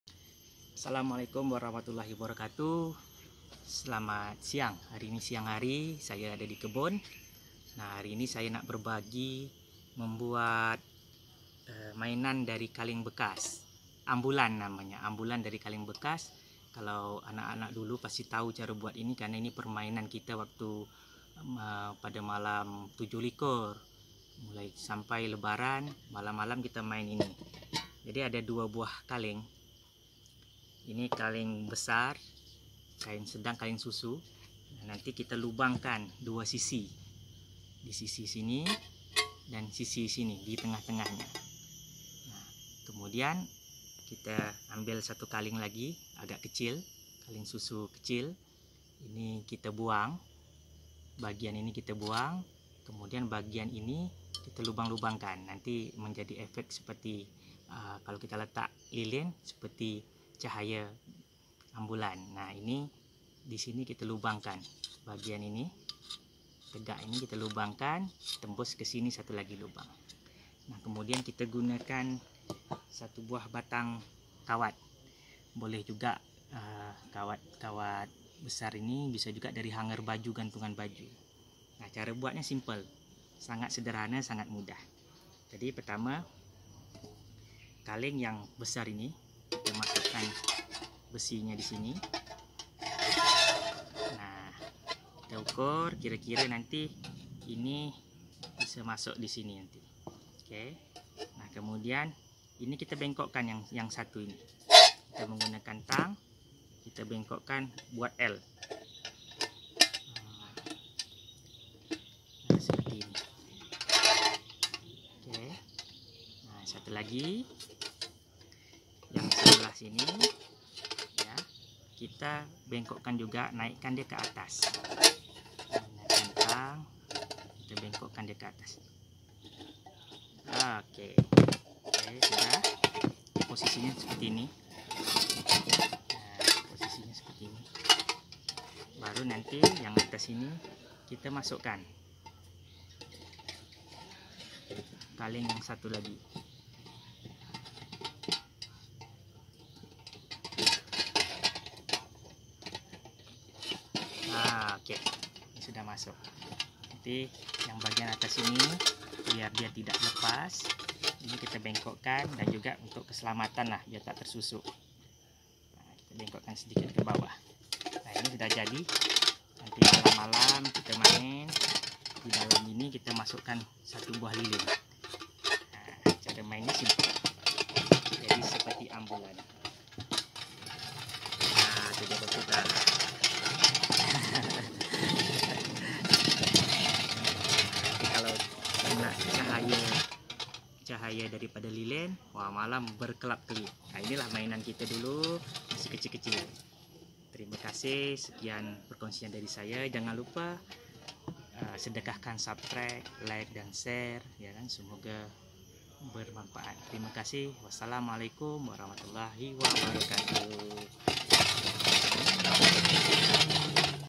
mainan ambulan dari kaleng susu bekas permainan anak-anak zaman dahulu pada bulan ramadhan
bahan bahan
2 buah kaleng susu
1 buah kawat panjang lebih kurang 40 cm

alat
paku pelubang kaleng
tank jepit
pembuka kaleng susu